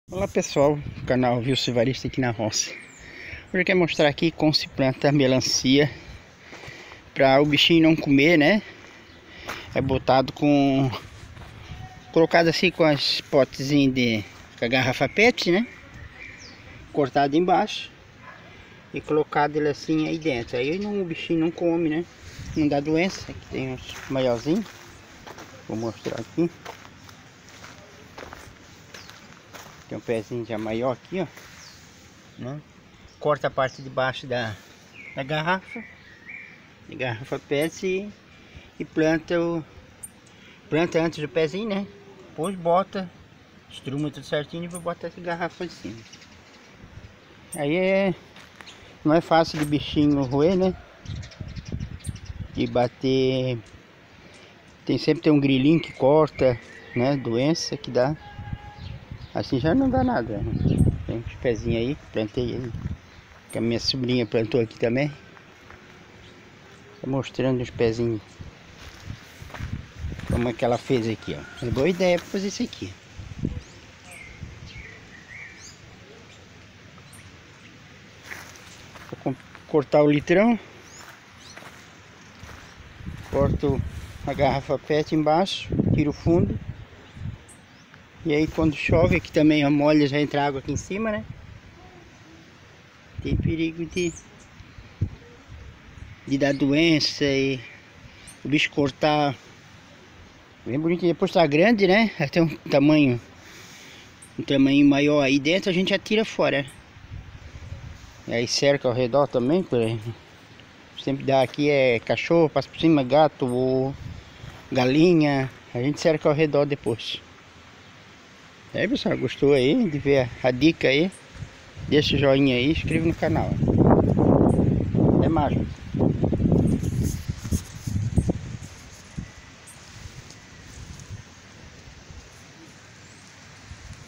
MANEIRA FÁCIL e SEGURA de PLANTAR MELANCIA
ESTE É UM MÉTODO SEGURA DE PLANTAR MELANCIA SEM PERDER NEM UM PE PARA AS PRAGAS E AS DOENÇAS

SE VC FEZ ALGUM VASO COMPARTILHA LÁ PARA TODOS VEREM

RECEITAS DA ROÇA,tudo  sobre a roça ,tutorial,receitas e dicas rural,Notícias sobre agricultura, Pecuária, empreendedorismo rural,

 MINHA CAIXA POSTAL; 30 CEP:88240000